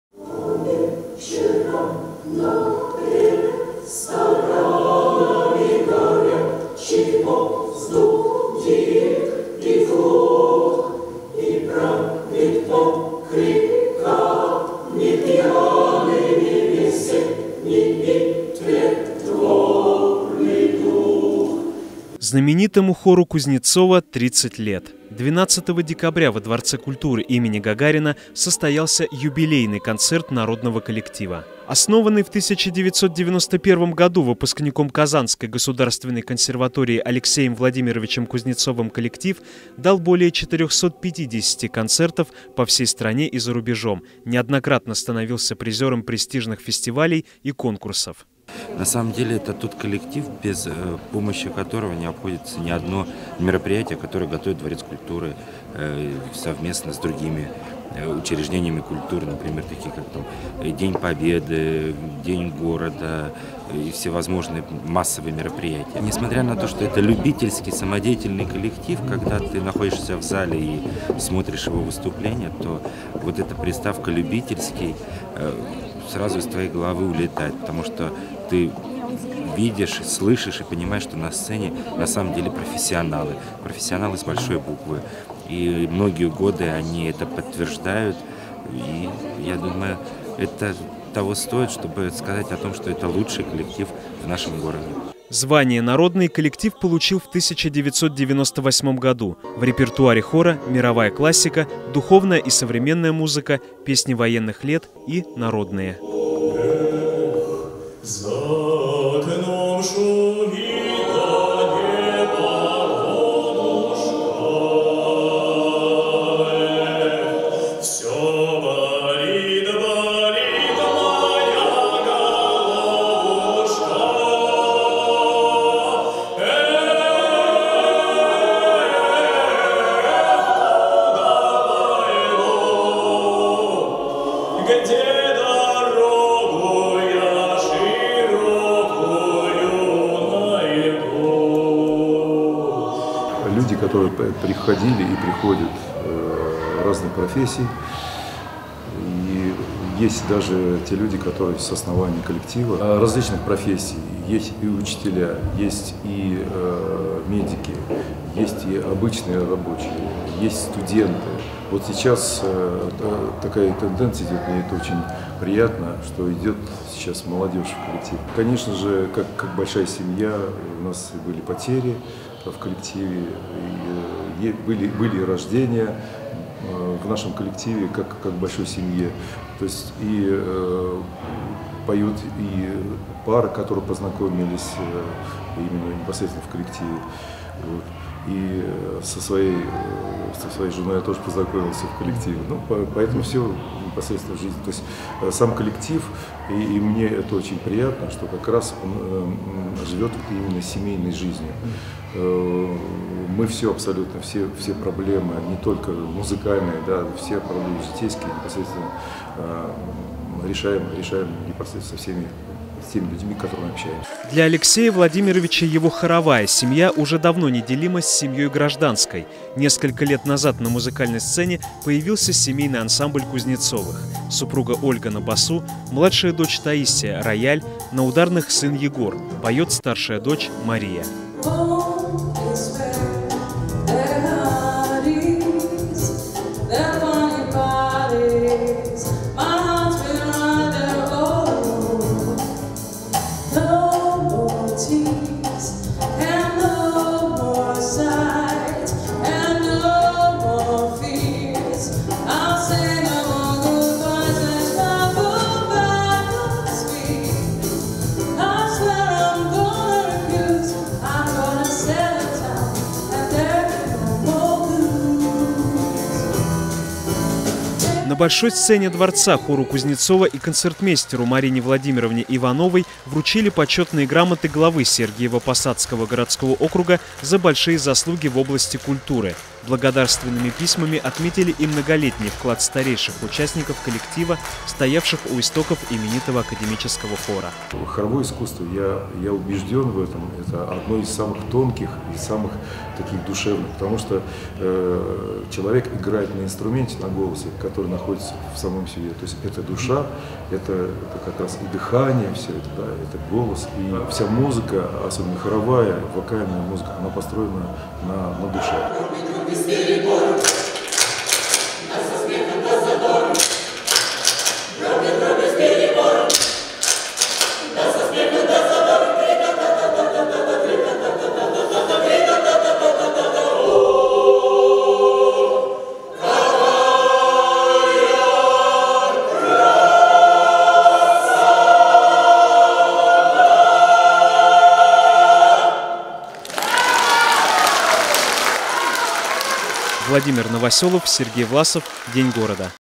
Юбилейный концерт хора под управлением Алексея Кузнецова
В этом году Академическому хору Дворца культуры имени Юрия Алексеевича Гагарина под руководством Алексея Владимировича Кузнецова исполнилось 30 лет. В честь своего юбилея 12 декабря прославленный коллектив дал концерт, где были исполнены лучшие песни из их репертуара.